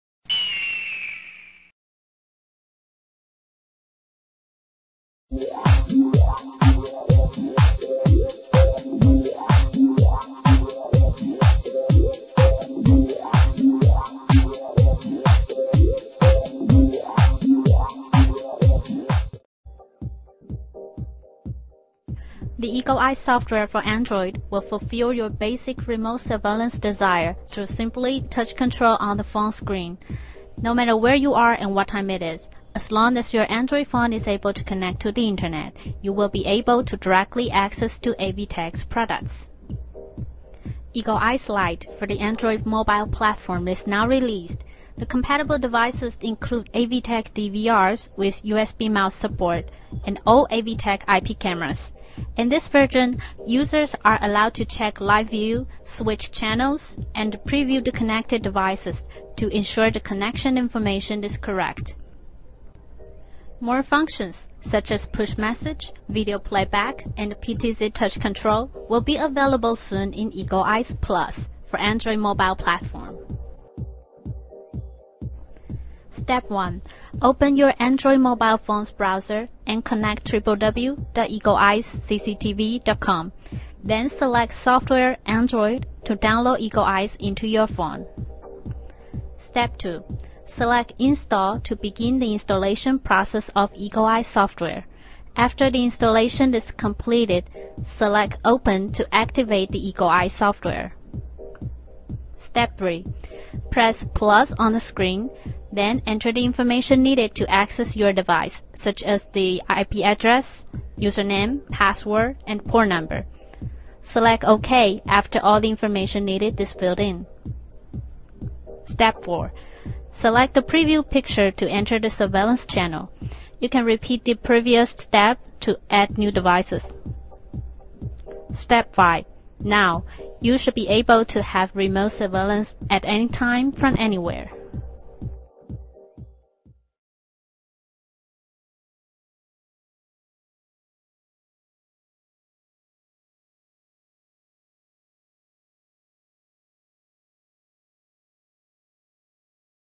Demonstratie van de installatie van Eagle Eyes op de Android telefoon
In dit demonstratiefilmpje wordt stap voor stap weergegeven hoe de installatie van EagleEyes op een Android telefoon gaat.